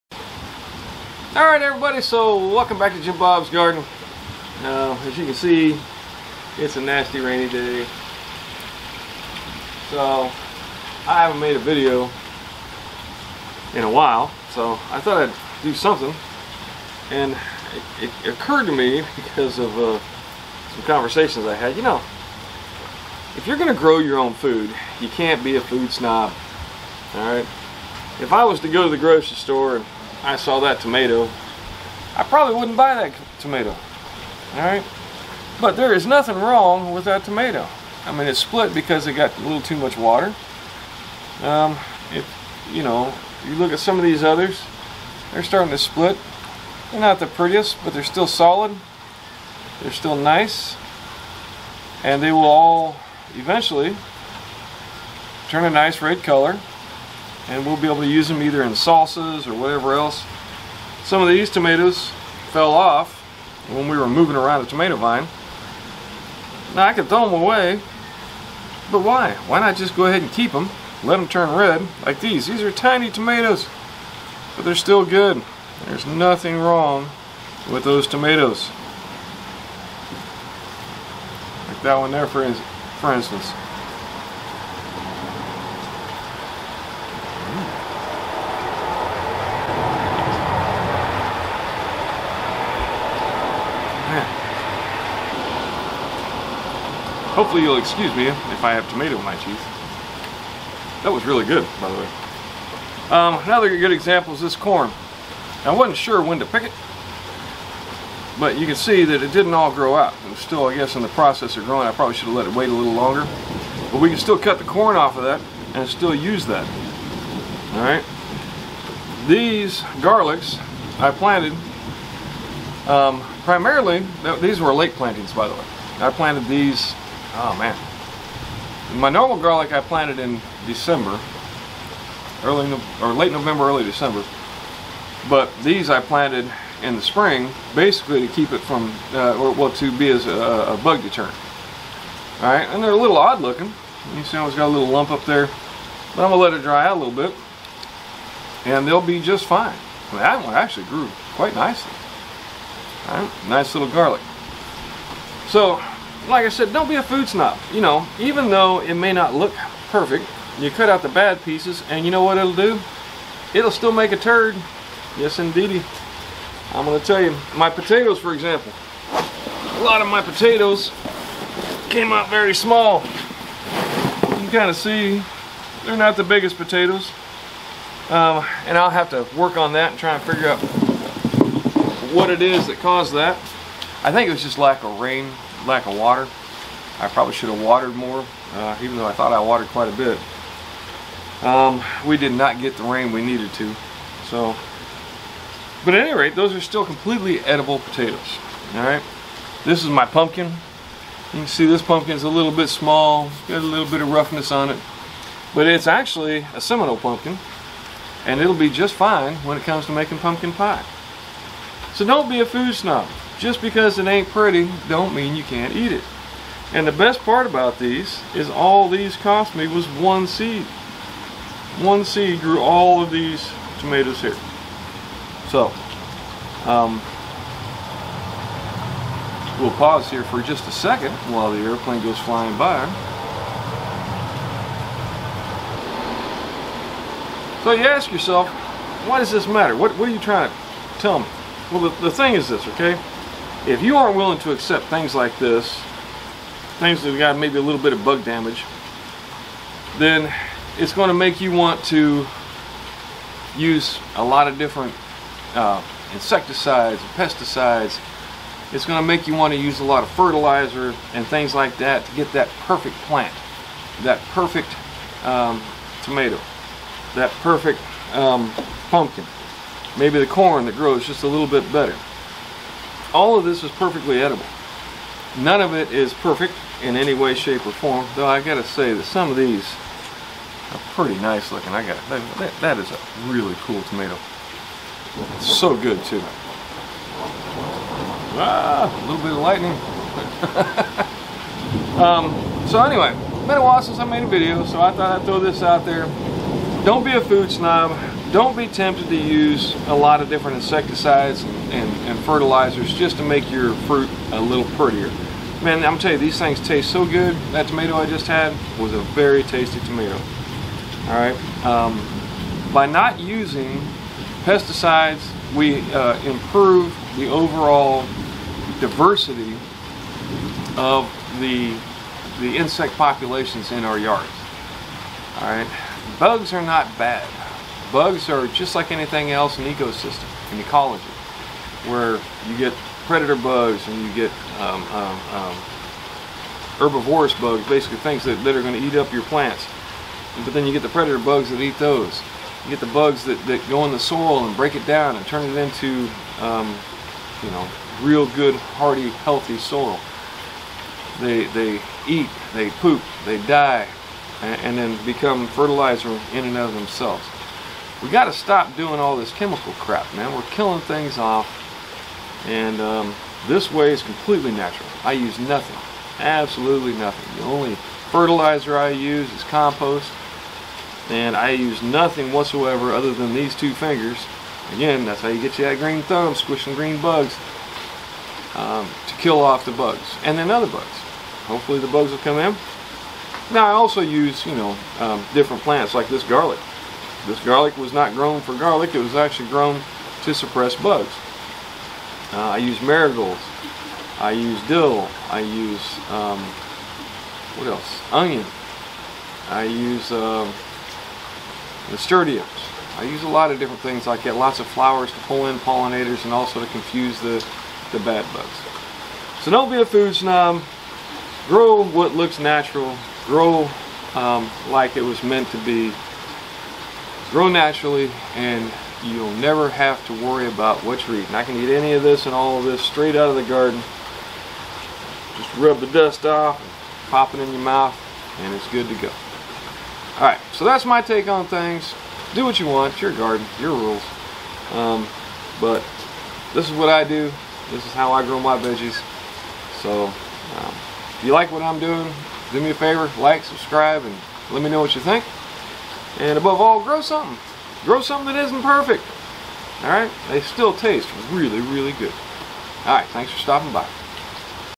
Don't Be a Food Snob
Some people try to grow the perfect crop.  To do this however it usually requires the use of chemicals to kill bugs and to improve growth.  The pursuit of perfection is part of the problem that has caused many problems with the environment and the huge loss of insect populations world wide.  This along with the cost of those inputs are some of the reasons that I do not use chemicals in my garden and why I only use compost to improve my food crops.